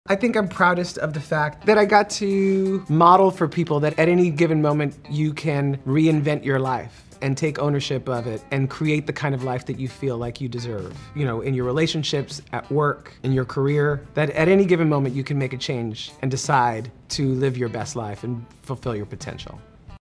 What Is Wilson Cruz Most Proud Of With Star Trek Discovery Ending?
StarTrekDiscovery star WilsonCruz reflects on what his is most proud of from his 5 year journey as HughCulber in the StarTrek series.